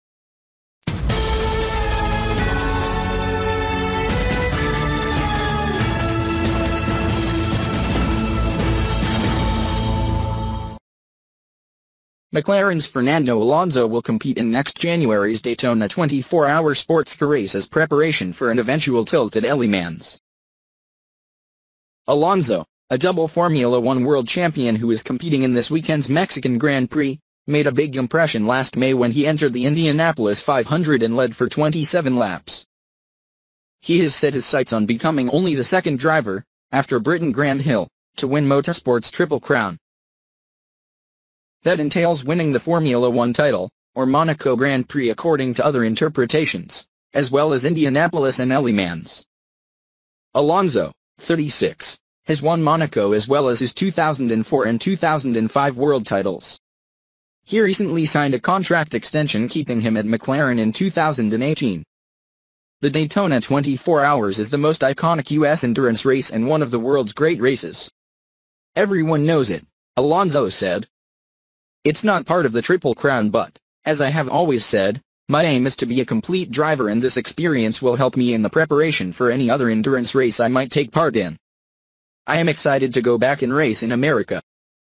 Alonso races at Daytona with eye on Le Mans
McLaren's Fernando Alonso will compete in next January's Daytona 24 Hour sportscar race as preparation for an eventual tilt at Le Mans.

Alonso, a double Formula One world champion who is competing in this weekend's Mexican Grand Prix, made a big impression last May when he entered the Indianapolis 500 and led for 27 laps.

He has set his sights on becoming only the second driver, after Briton Graham Hill, to win motorsport's Triple Crown.